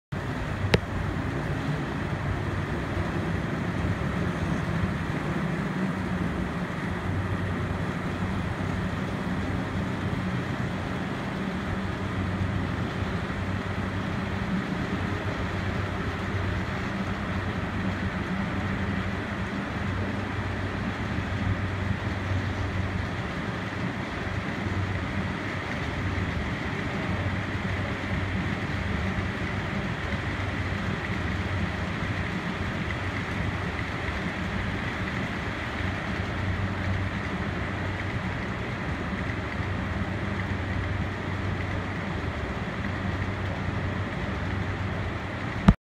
CSX reefer train on the Illinois & Western
A six unit CSX reefer train flies over the high line above the Illinois & Western locomotive shop at the Elmhurst Model Railroad Club.